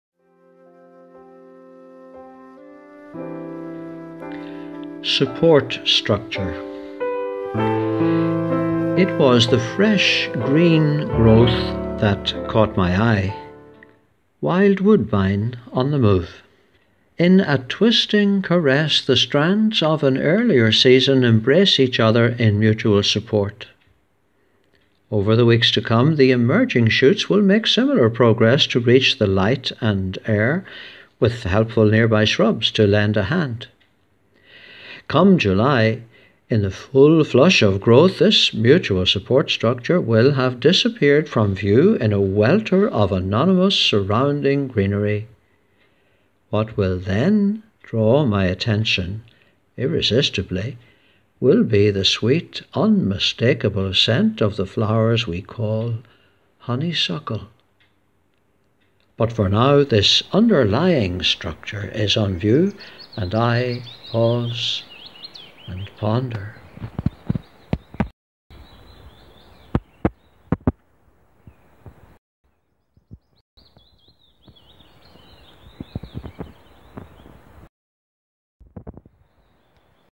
Pause and Ponder along the Pilgrim Path - 'Support Structure'
A weekly reflection with Fr La Flynn from along the Pilgrim Path at Lough Derg. This week's reflection - 'Support Structure'
It was the fresh green growth that caught my eye:
wild woodbine on the move.
In a twisting caress the strands of an earlier season
embrace each other in mutual support.
Over the weeks to come the emerging shoots
will make similar progress to reach the light and air,
with helpful nearby shrubs to lend a hand.

Come July, in the full flush of growth
this mutual support structure
will have disappeared from view
in a welter of anonymous surrounding greenery.
What will then draw my attention irresistibly
will be the sweet unmistakeable scent
of the flowers we call honeysuckle.

But for now this underlying structure is on view,
and I pause and ponder.

Fr La Flynn
Prior, Lough Derg